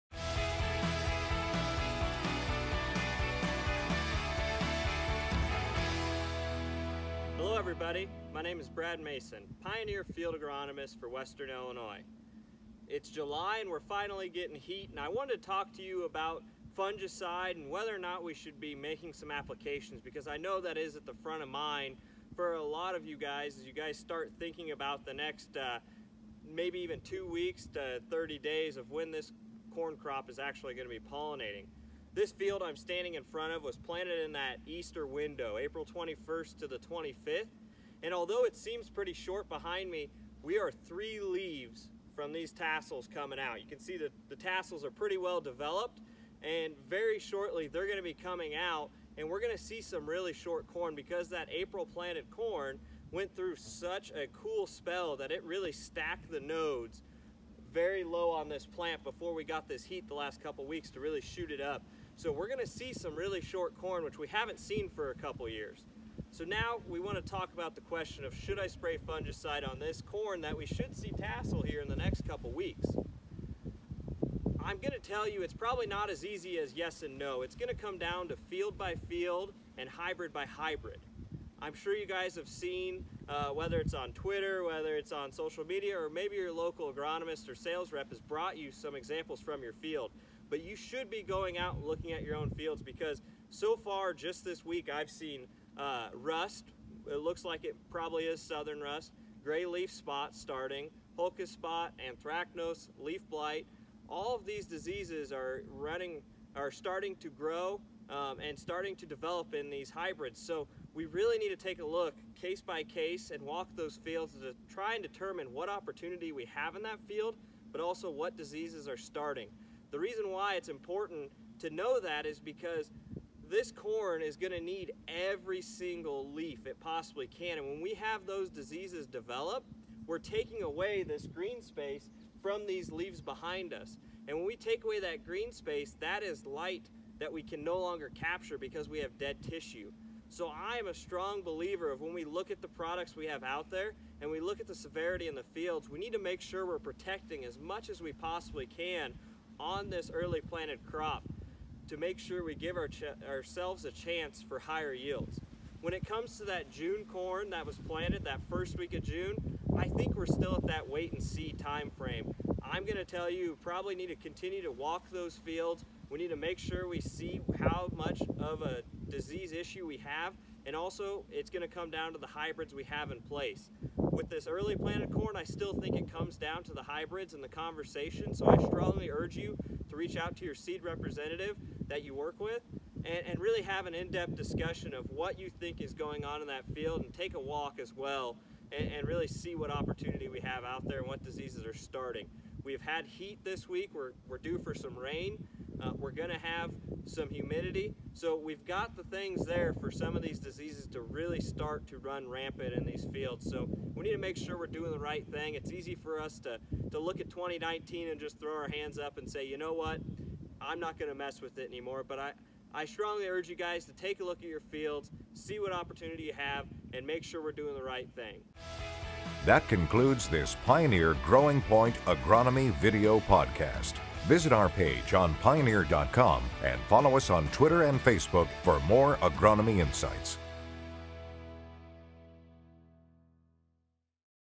Fungicide and Disease Discussion
Is not the right time to apply foliar fungicides on your fields? Pioneer Field Agronomist Bradley Mason breaks down the factors to consider before you apply.